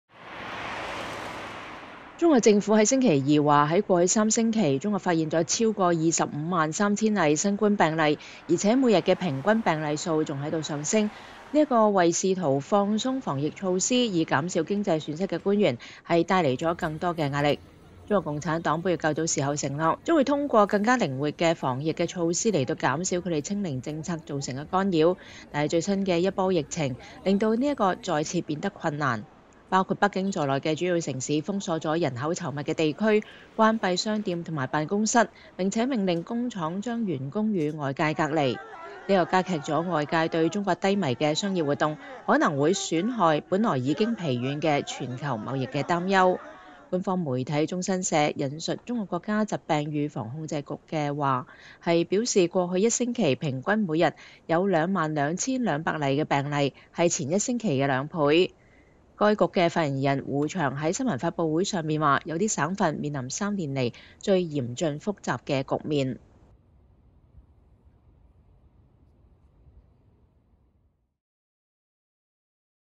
中國防疫措施再度加劇對全球經濟影響的擔憂
中國政府週二(11月22日)表示，在過去三週內，中國發現了超過25萬3千例新冠病例，而且每天的平均病例數還在上升，這給試圖放鬆防疫措施以減少經濟損失的官員帶來了更多壓力。